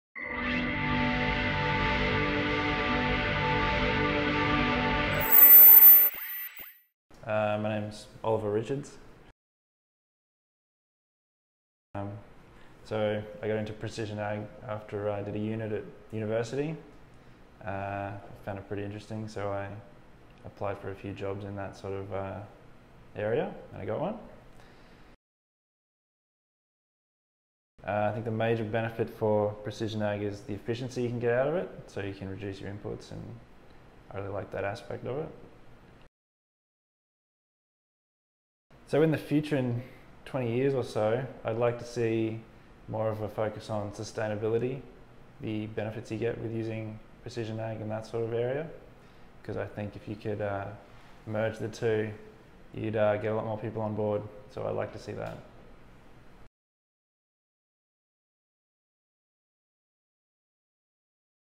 Oliver Richards
Interview with Oliver Richards from Ag Leader Technology, conducted by Adrian Roles for the Precision Ag CoP at the Ag Leader Dealer Conference 2019.